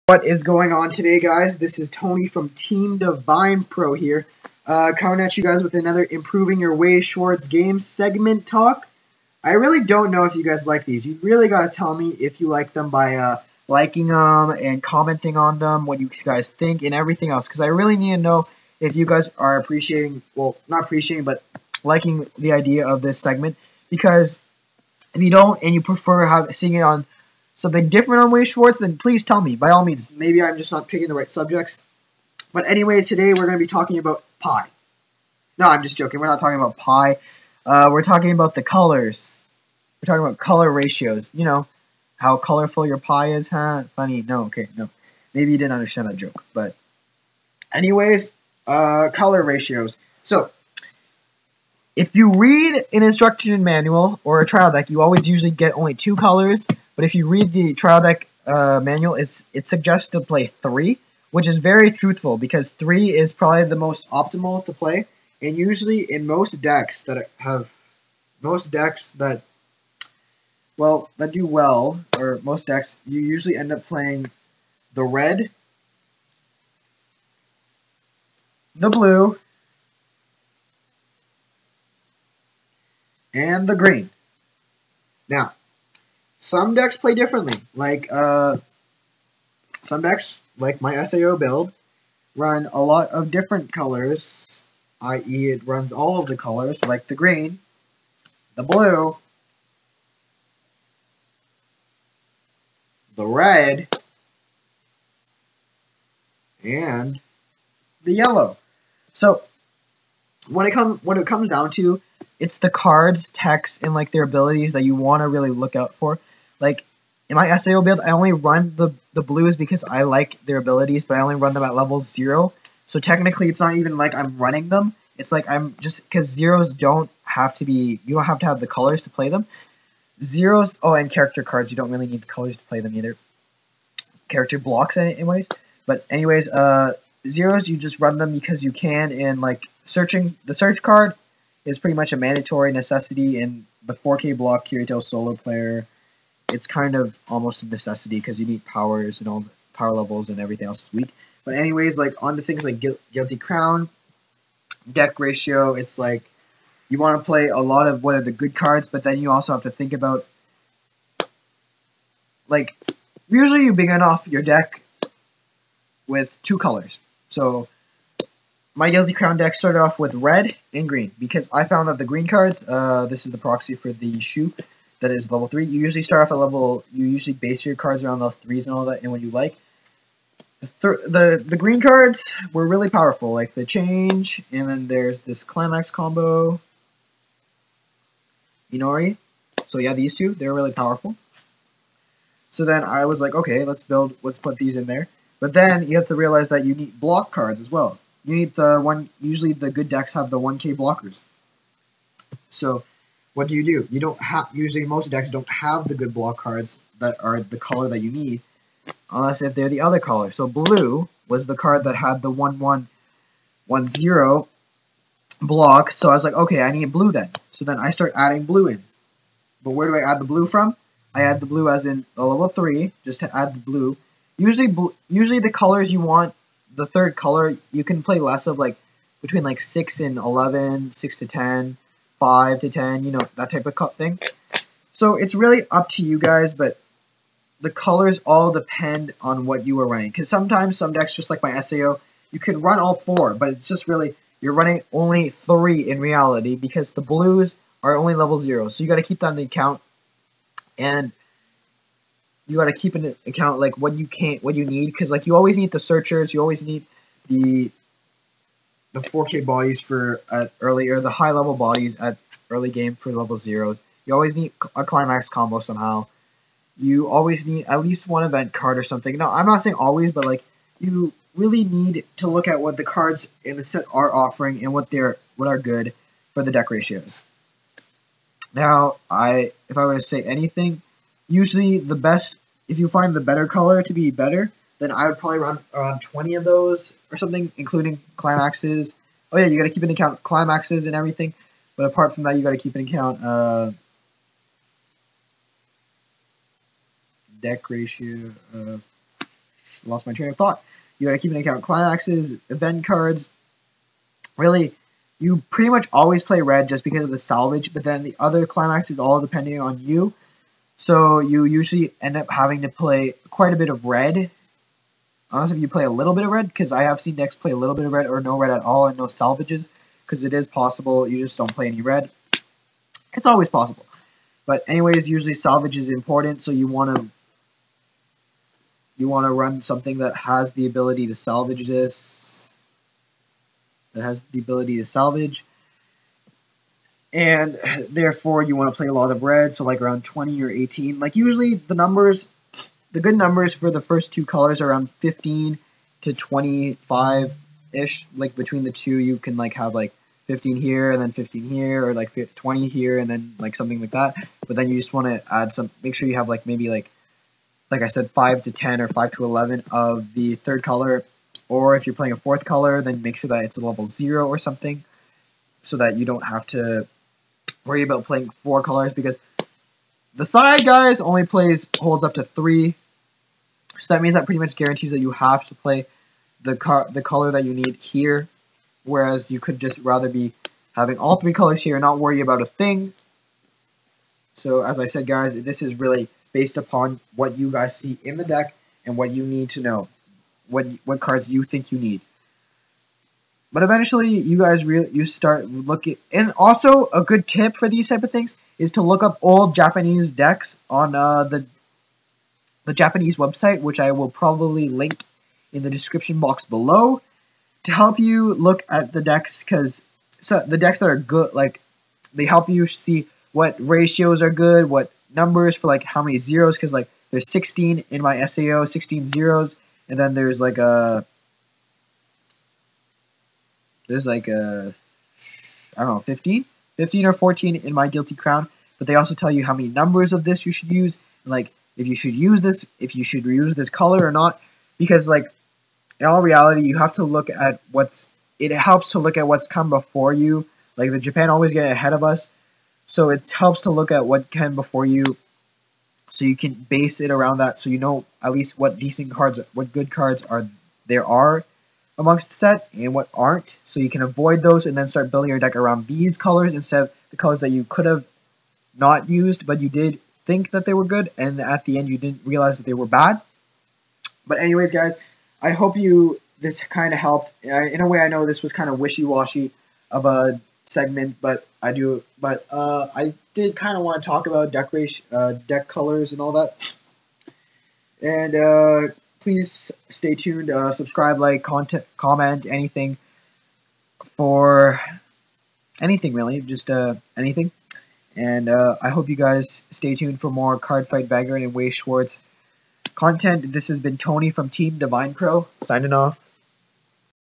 Improving your Weiss Schwarz Game Ep 5 - PIE????.......(Deck Colour Ratios)
So talking today about a highly crucial subject when building your deck - Colour Ratios.